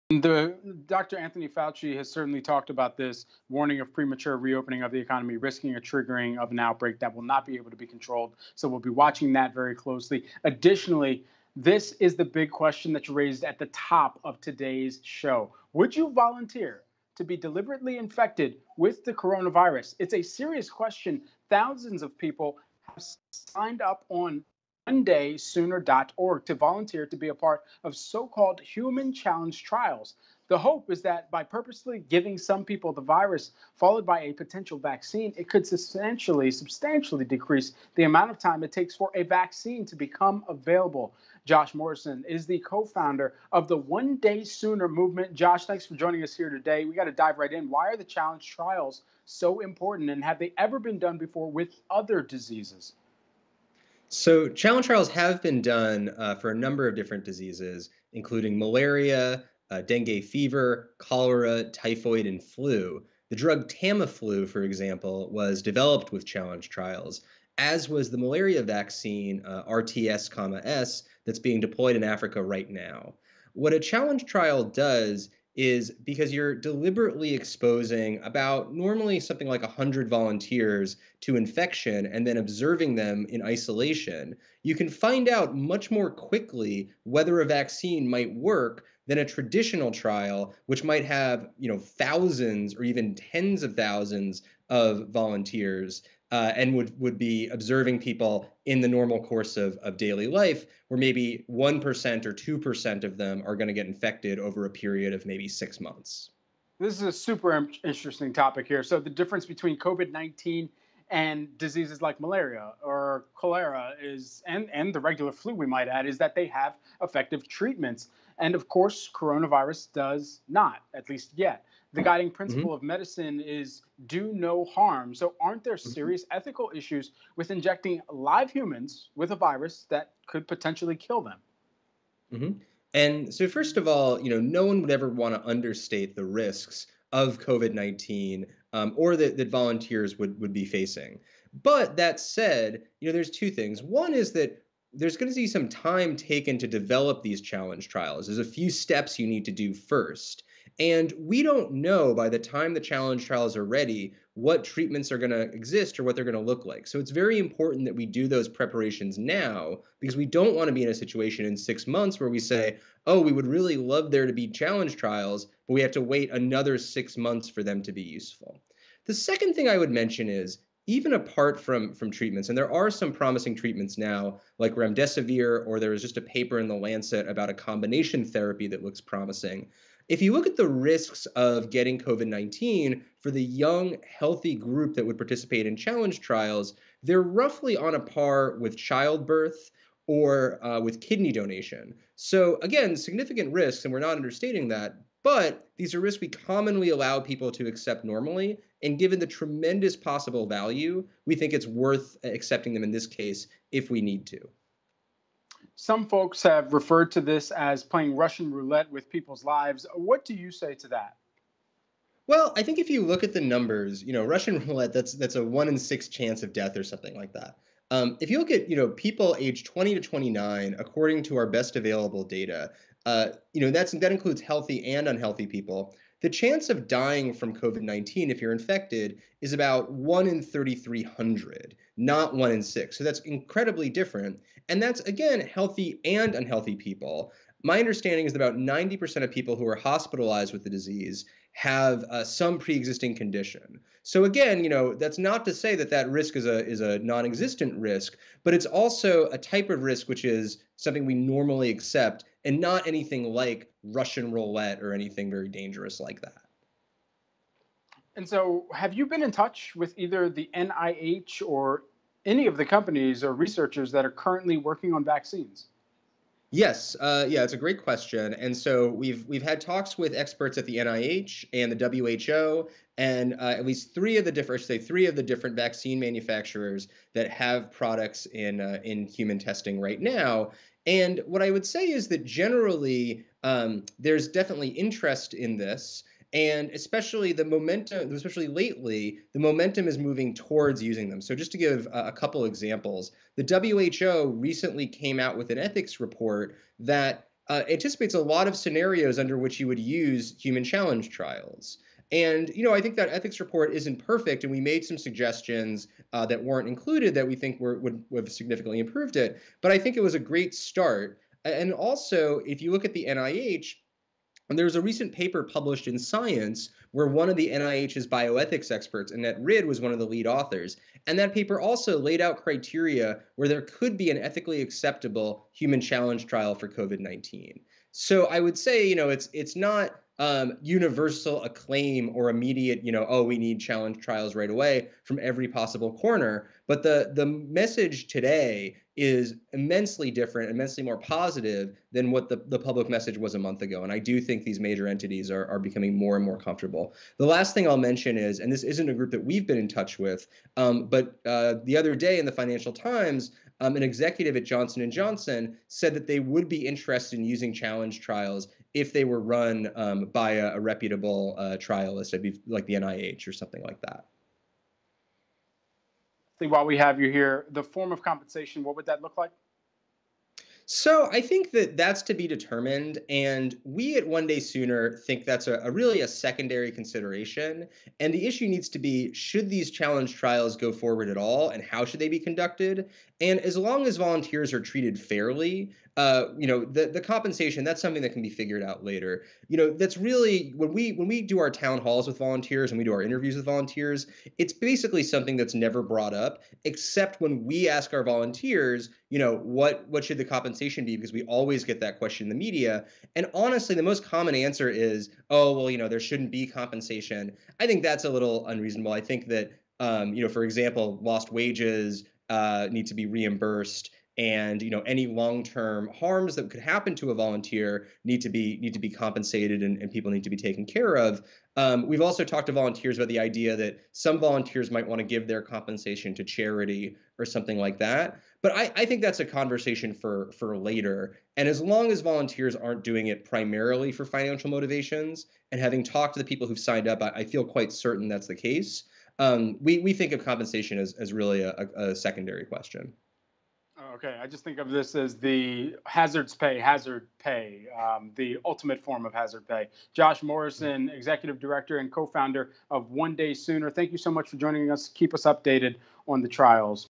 Search for a Cure: Human Challenge Trials for Coronavirus
Josh Morrison, Co-Founder of the One-Day-Sooner Movement, talks to Cheddar's Brad Smith about human challenge trials followed by a potential Coronavirus vaccine.